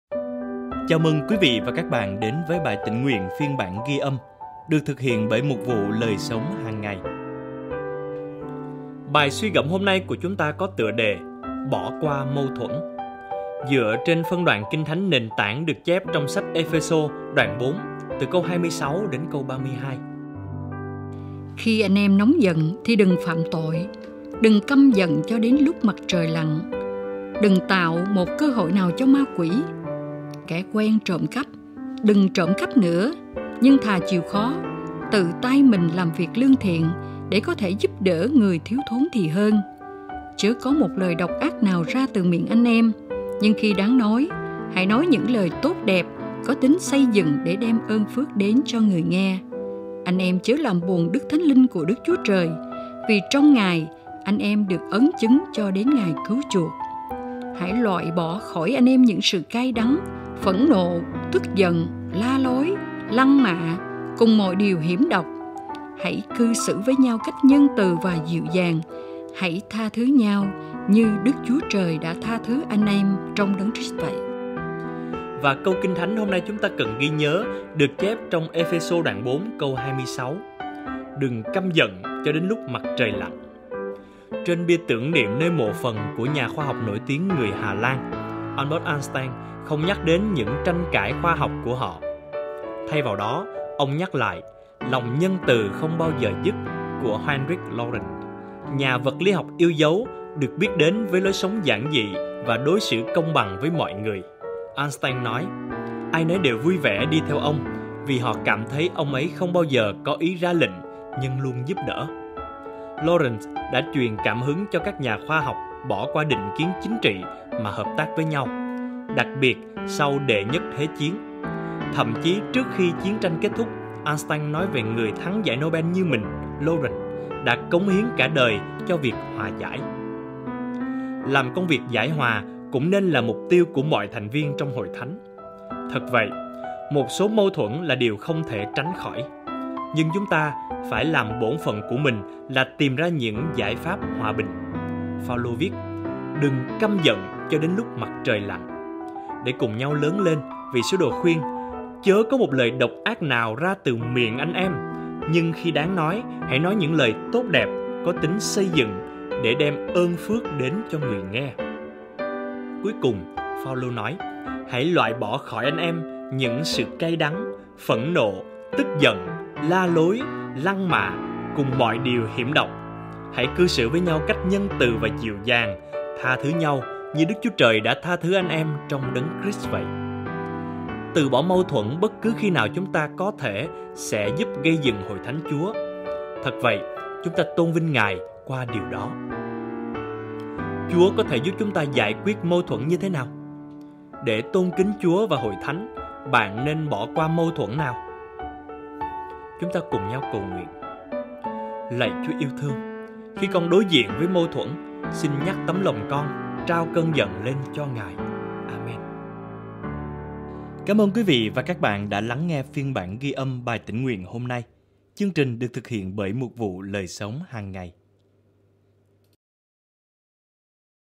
Bỏ Qua Mâu Thuẫn - ODB 20 Nov 2020
Lạy Chúa yêu thương, khi con đối diện với mâu thuẫn, xin nhắc tấm lòng con trao cơn giận lên cho Ngài.